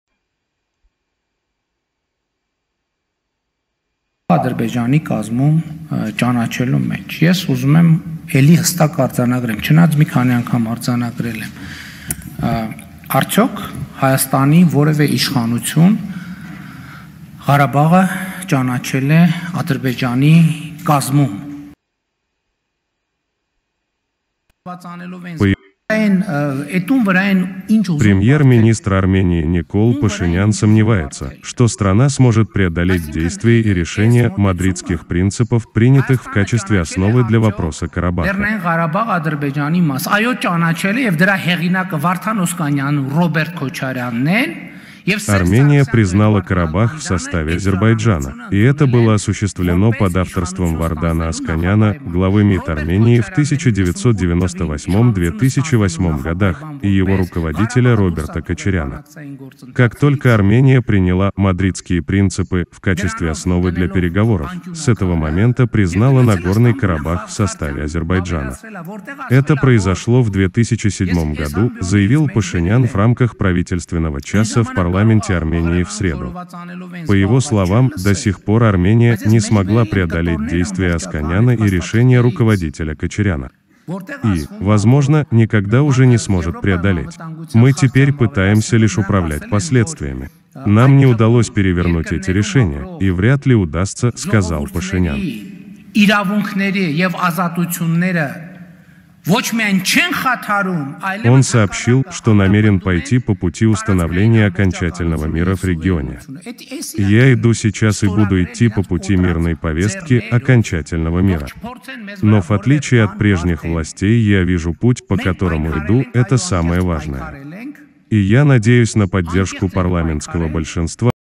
Я взял на себя эту ответственность и иду по этому пути – Пашинян заявил о реализации «повестки мира»
Источник: Ютуб канал "boodro news"